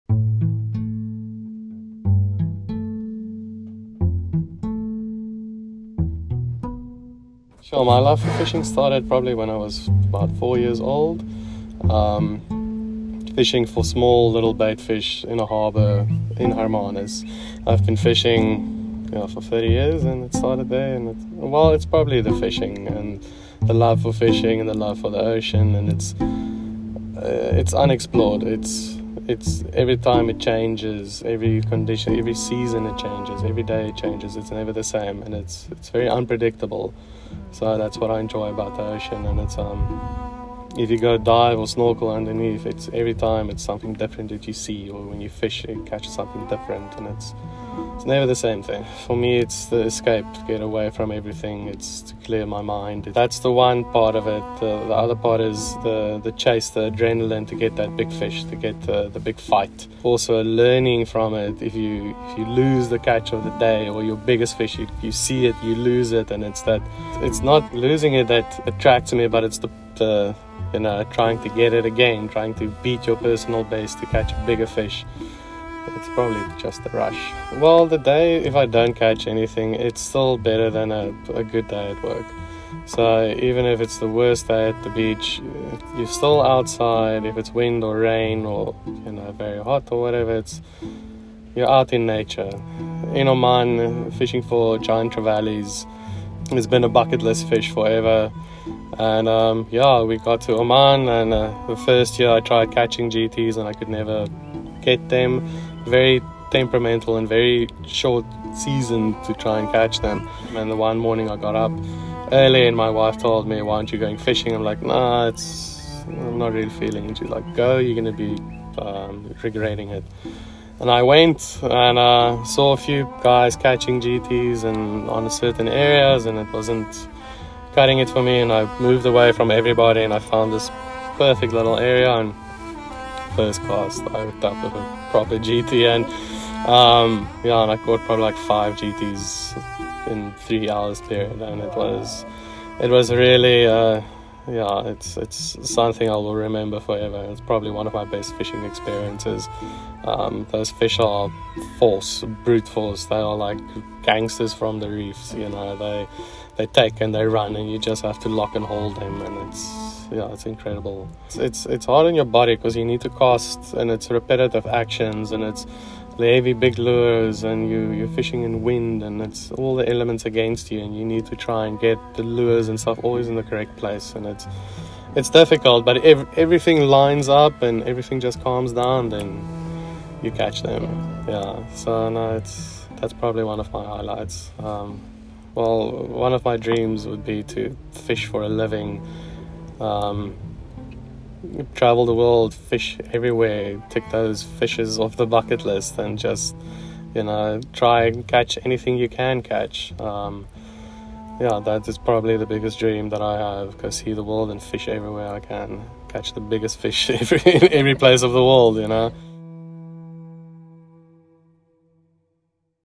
Fishing For Life - TheBeardedFisher
A short video I recently did with @vanartist talking about the ocean and why I love fishing.

Thanks to Wrangler x ATG for the fishing apparel